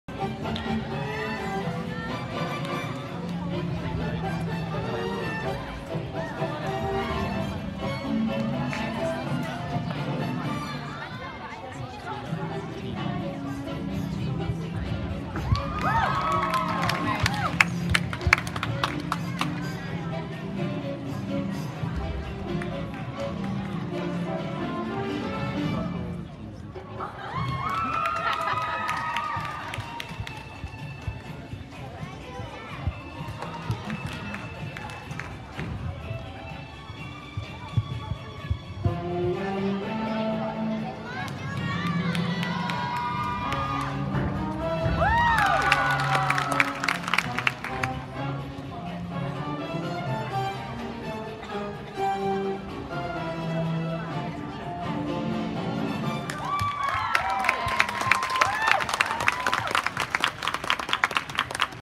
SEASAC Gymnastics 2016 ~ Julia Sipelis Level 5 FX 9.175 6th Place
UWCSEA East Dragons Gymnastics